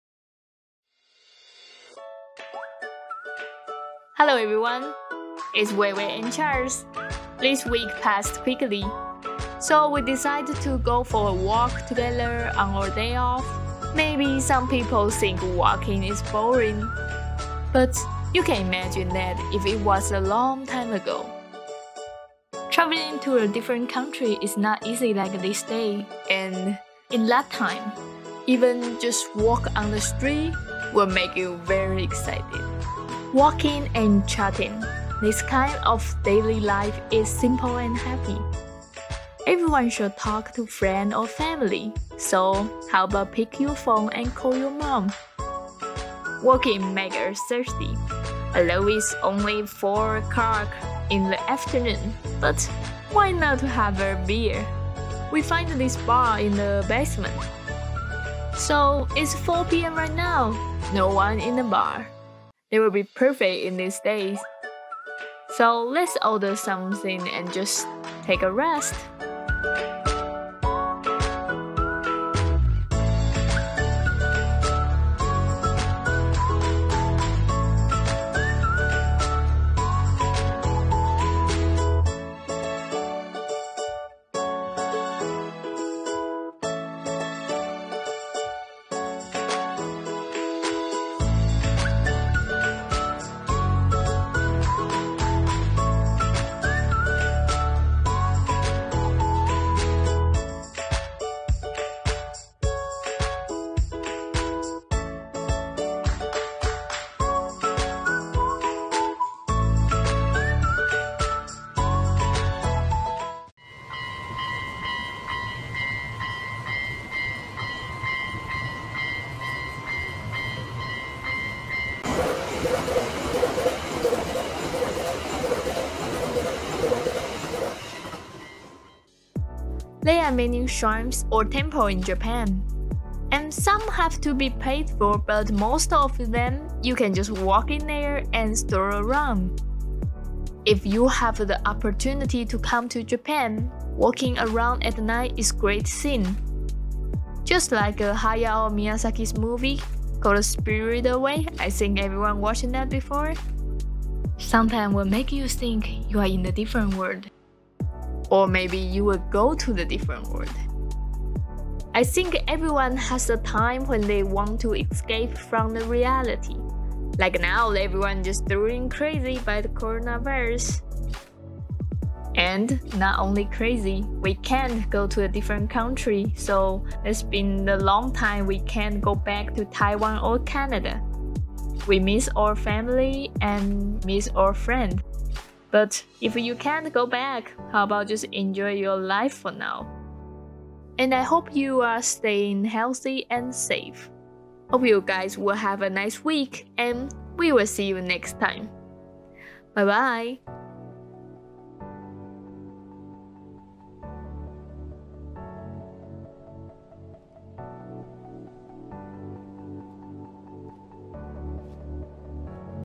Walking in Japan: Cherish the Moment
Hi~Everyone! This week passed very quickly! Hope everyone is healthy and safe!
Today's video will show you the streets of Japan, I hope you will like it !